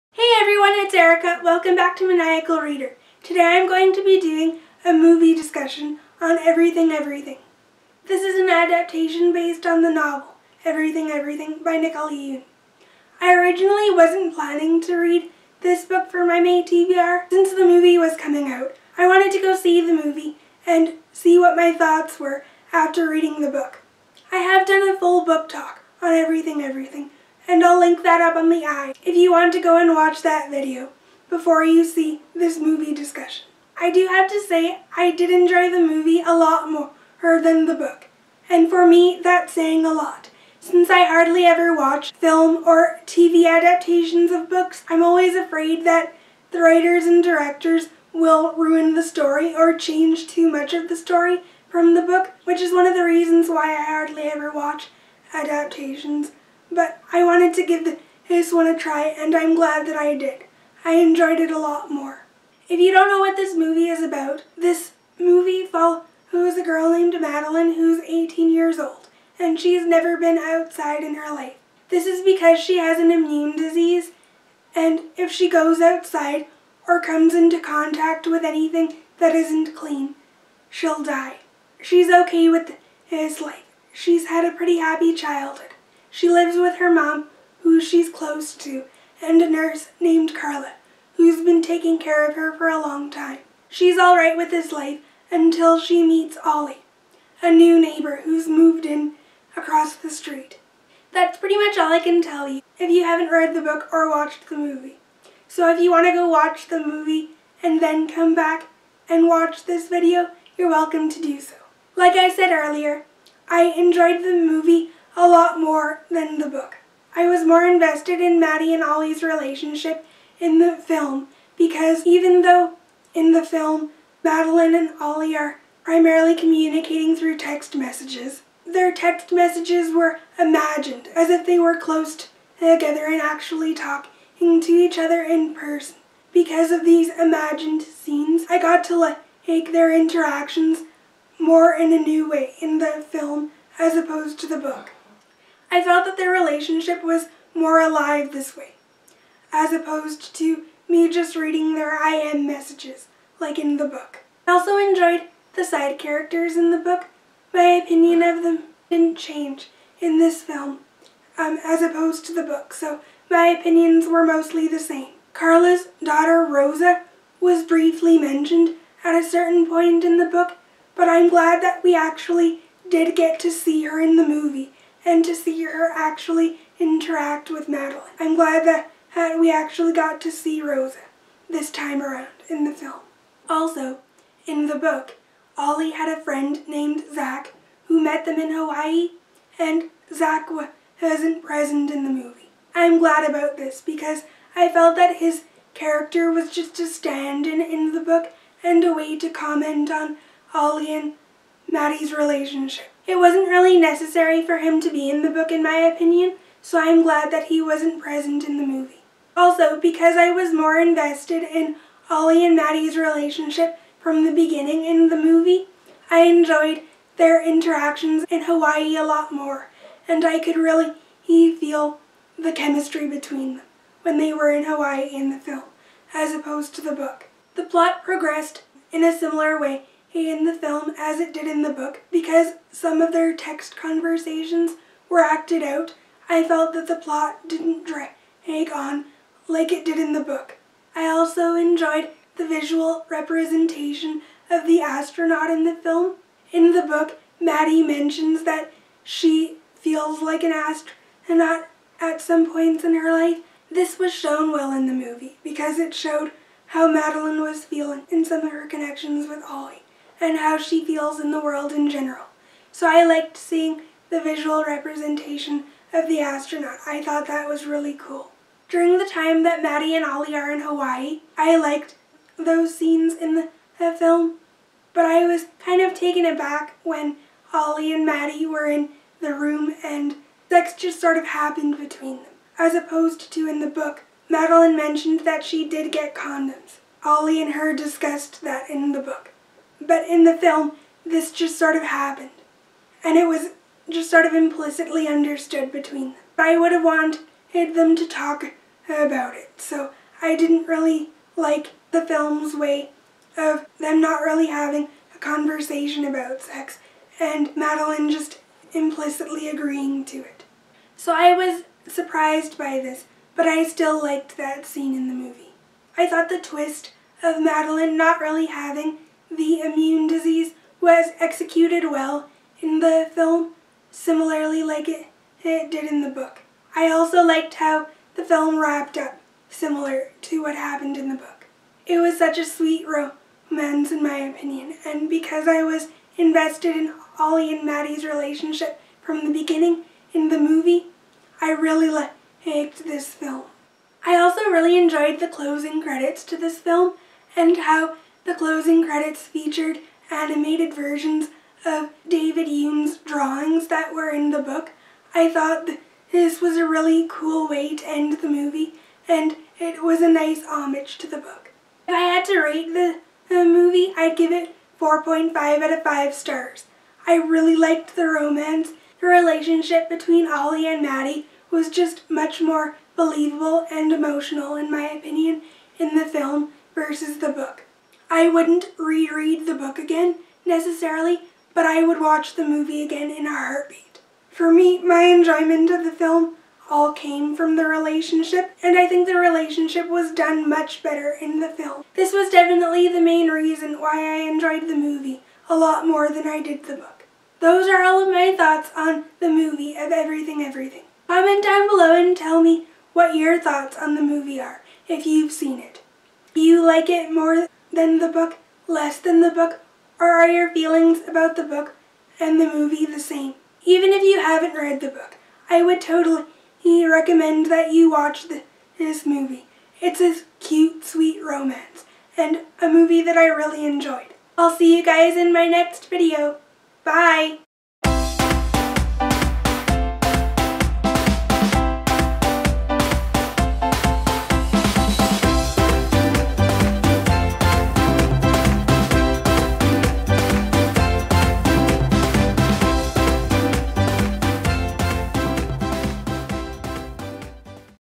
Everything, Everything | MOVIE DISCUSSION
This is my discussion on the film. I hope you enjoy. There will be spoilers.